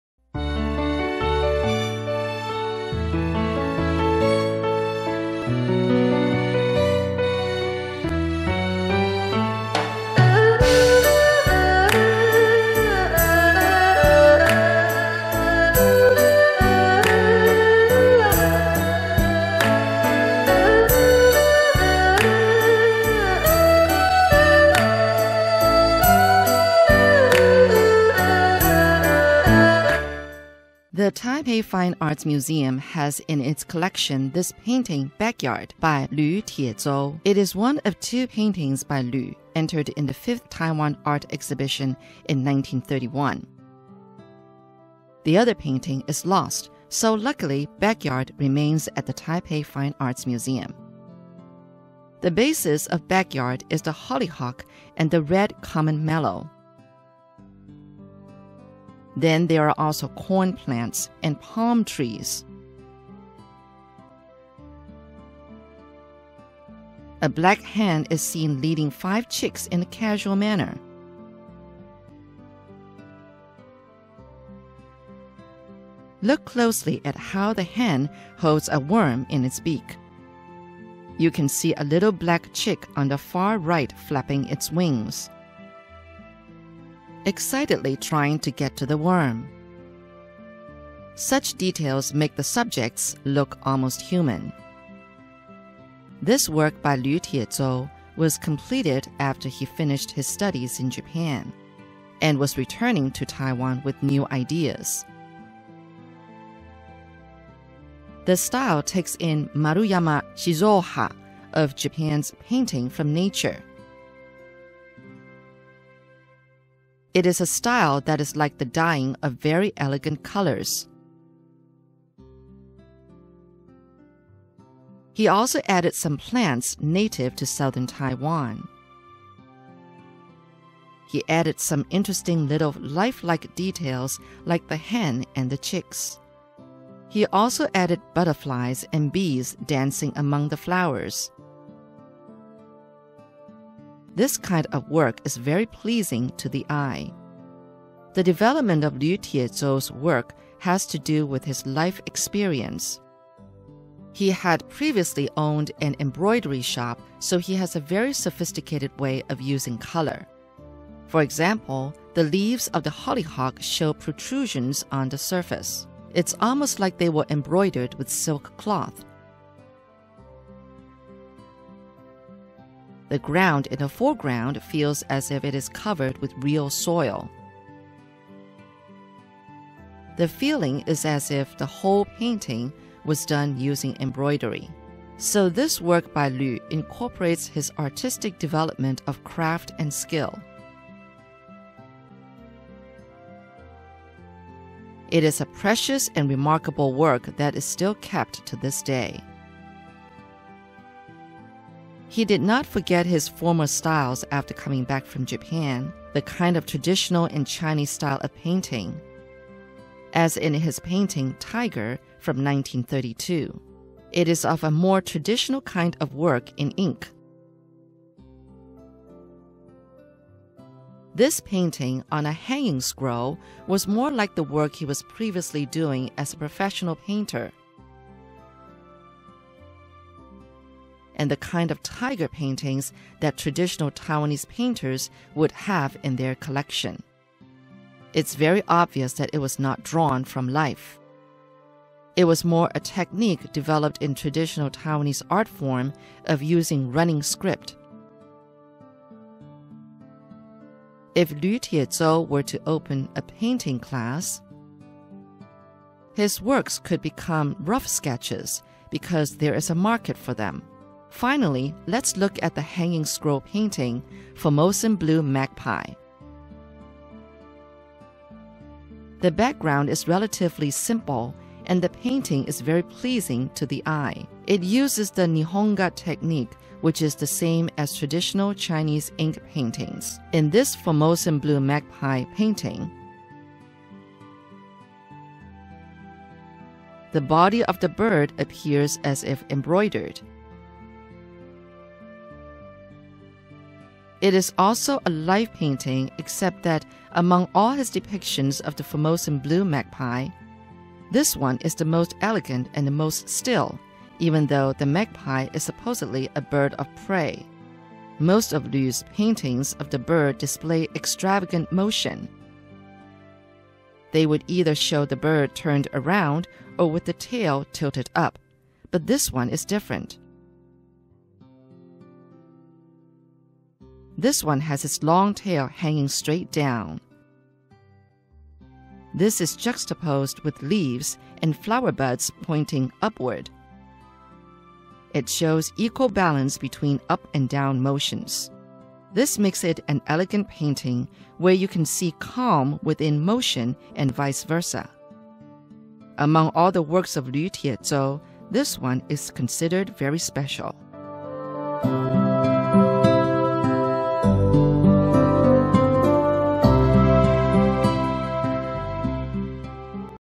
【RTI】Stories Behind the Art (21) Lu Tieh-chou～Backyard
Lu Tieh-chou was born in 1899 in the Daxi district of Taoyuan city. After graduating from Daxi Public School in 1913, he went on to enroll at the Taiwan Governor General Office Industrial Institute but took a leave of absence after two years. In 1920, he served as a member of the council in charge of Daxi Street affairs. In 1923, Lu moved to Dadaocheng and opened an embroidery shop. He went to Kyoto, Japan in 1928 to further his artistic studies before returning to Taiwan two years later. Lu died in 1942.